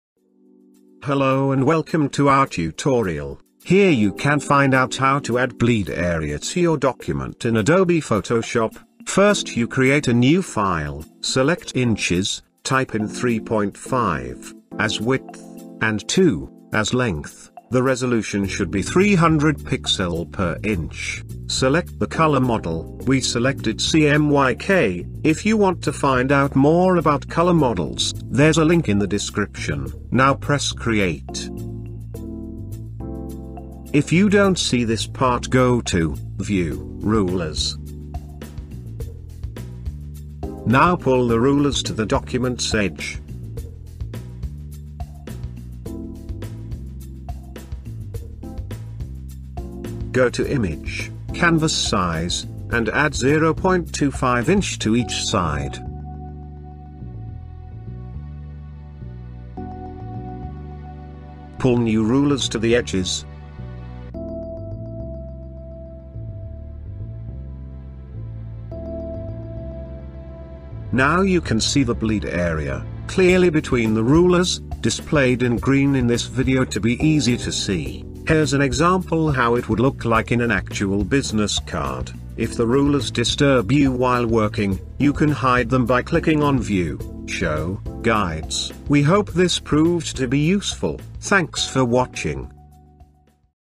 How to add bleed to business card in adobe photoshop cc 2017
In this video you can find out how to make the bleed area visible in adobe photoshop easily in a few minutes with the help of rulers.

If you want to find out more about color models,

If you want to find out more about bleed,